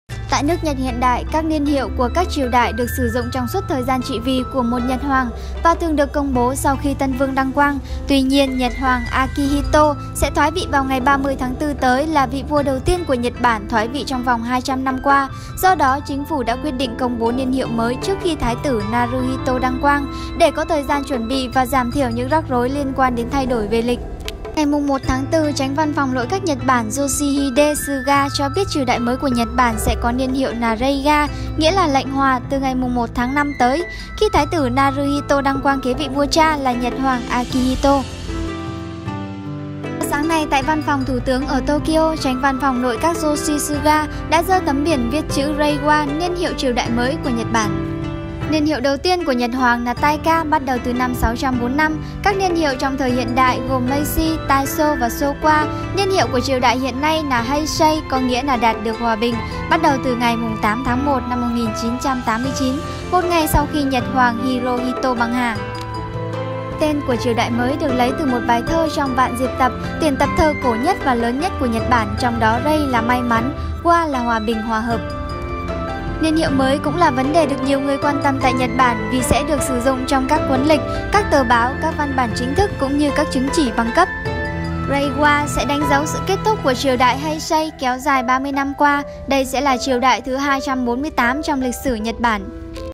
Tin Nóng Từ Nhật Bản: Nhật Hoàng Đã Chính Thức Thoái Vì | Nhật Bản Bước Qua Một Triều Đại Mới
📍 Địa chỉ: 1806/120/1 Huỳnh Tấn Phát, Huyện Nhà Bè, TPHCM